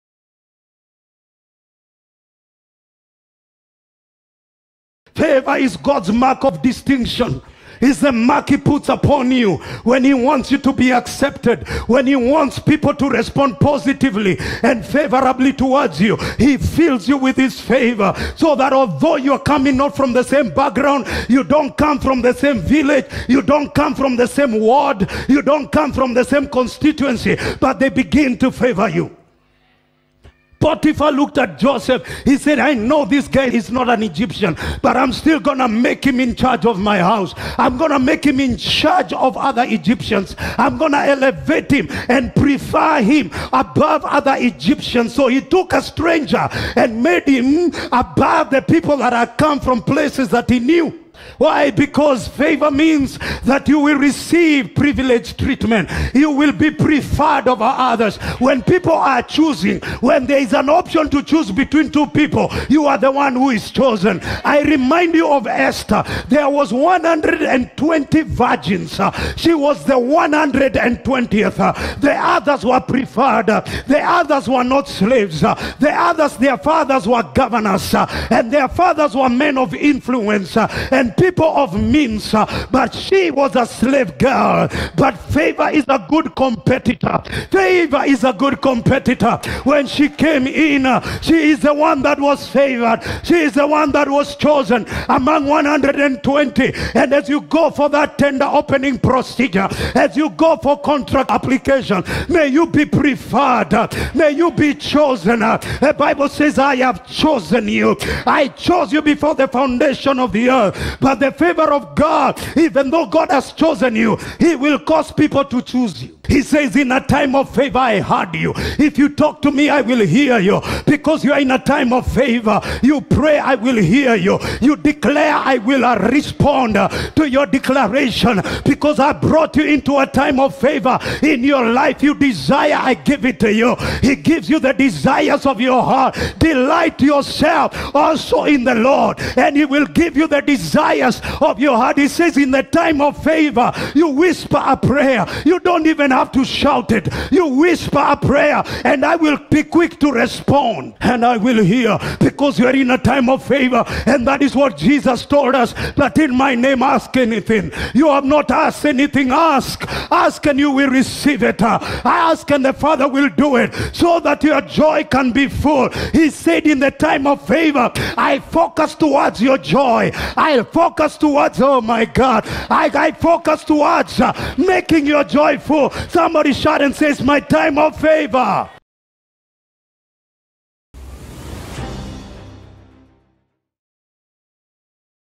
The Favor of God causes you to operate above your qualification! - Pastor Morris Gacheru
PAYBILL: 669611 | A/C: JCC